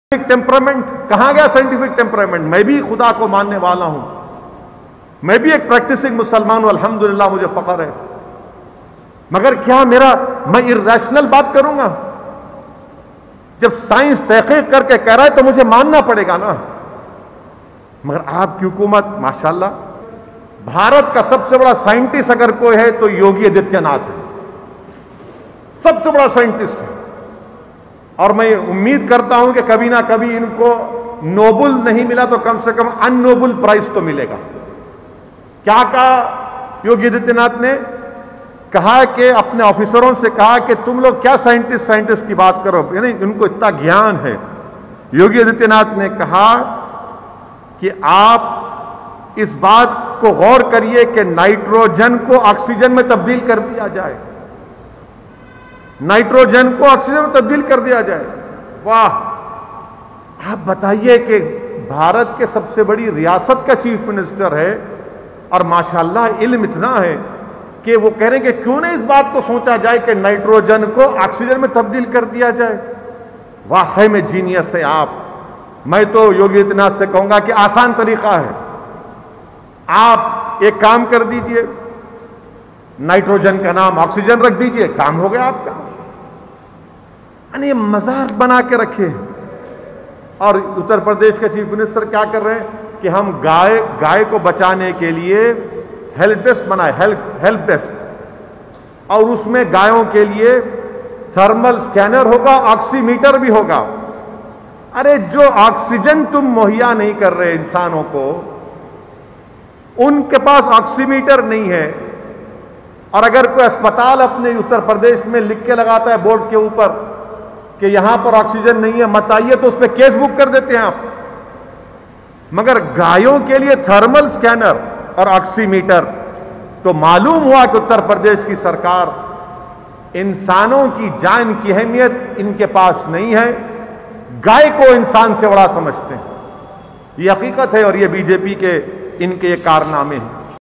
NITROGEN KO OXYGEN MAI BADLEGA YOGI ADITYANATH BHARAT KE SABSE BADE SCIENTIST HAI :ASADUDDIN OWAISI
NITROGEN KO OXYGEN MAI KAISE BADLEGA
CM YOGI ADITYANATH BHARAT KE SABSE BADE
SCIENTIST HAI : ASADUDDIN OWAISI